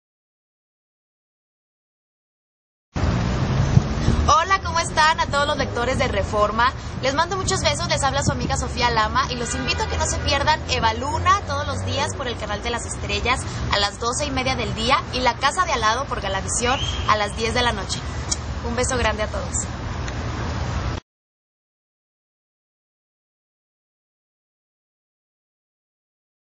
Saludos Reforma Gente - Sofia Lama
Luego de vivir cuatro años en Miami y haber participado en cinco telenovelas, Sofía Lama se ha impuesto la meta de trabajar en México, donde actualmente se transmiten dos melodramas que grabó en Estados Unidos: La Casa de al Lado y Eva Luna.